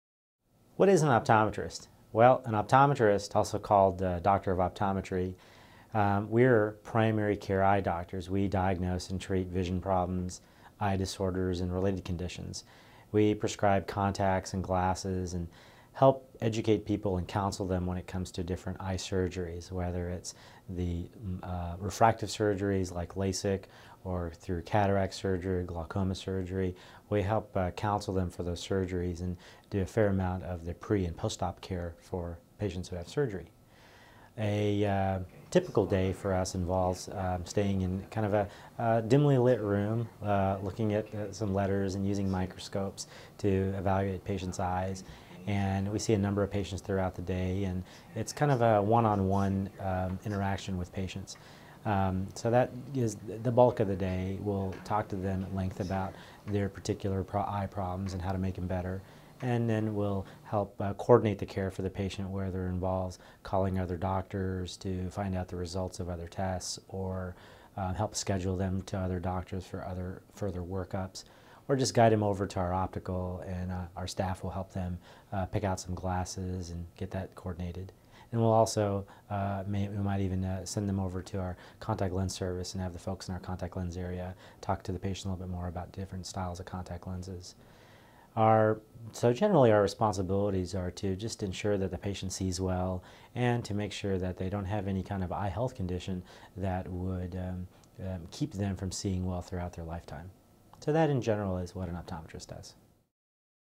Optometrist Career Information : Optometrist Job Description
An optometrist's job description includes diagnosing and treating vision problems and eye disorders, as well as prescribing contacts and lenses. Hear a practicing optometrist describe his job in this free video on career information.

Expert: Dr. Raj Patel
Bio: Dr. Raj Patel graduated as a member of the Gold Key International Optometric Honor Society with a doctor of optometry degree from the University of Houston.